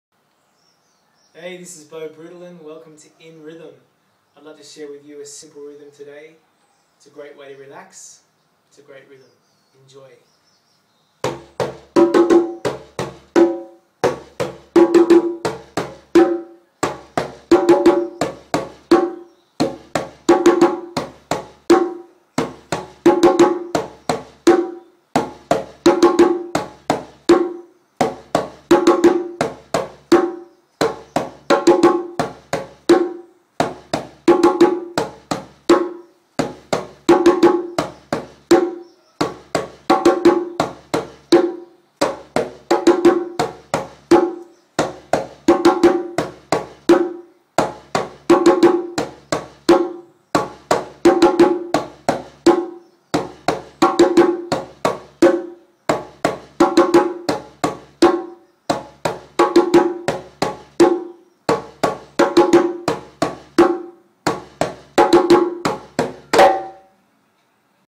Djembe drum lessons InRhythm 8
A cool groove for you to enjoy. This is a great rhythm to play while sitting in your office. Take 5 minutes to destress and refresh yourself. It will help you get some great ideas